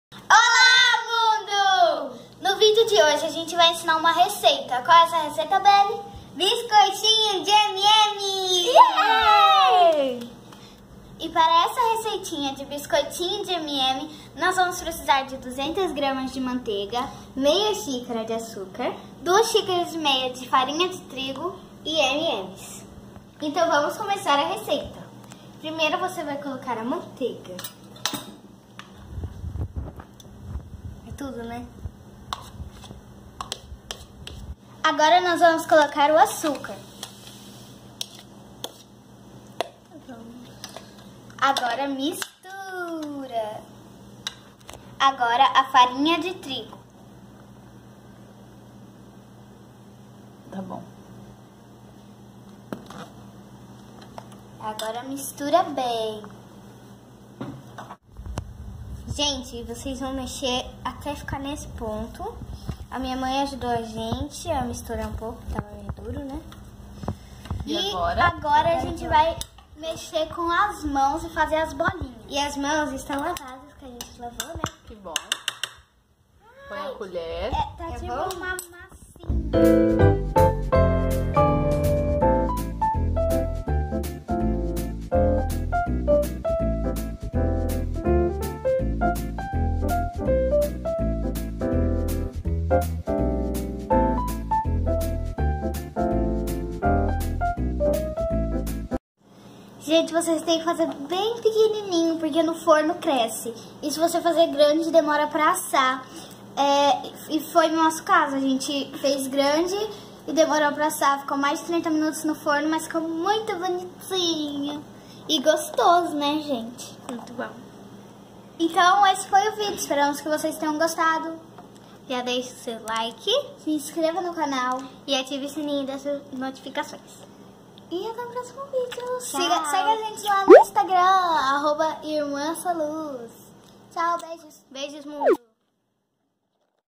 Biscoitinho de M&Ms
Receita ( créditos Dica da Naka)

100g açúcar refinado ou (1/2 xícara chá)

200g margarina sem sal (Temp. Ambiente)

300g farinha de trigo ou (2 ½ xícaras chá)

1 pacote de m&m´s

Em uma tigela misture a manteiga, o açúcar e a farinha de trigo aos poucos até formar uma massa homogênea.  Faça pequenas bolinhas e coloque um m&m´s no centro. Em seguida, acomode os biscoitos em uma forma retangular sem untar.

Preaqueça o forno a 180°C e asse por aproximadamente 15 minutos ou até dourar as bordas. Deixe esfriar e retire da forma. Armazene em pote com tampa para que preserve a sua crocância.